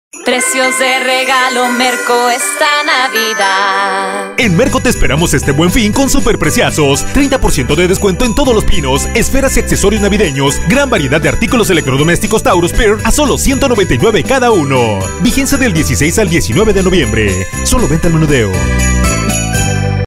El Buen Fin y los Precios de Regalo en Merco . Vigencia: del 16 al 19 de Noviembre 2018
En Merco te ofrecemos Preciazos del Buen Fin en accesorios Navideños, electrodomésticos y más.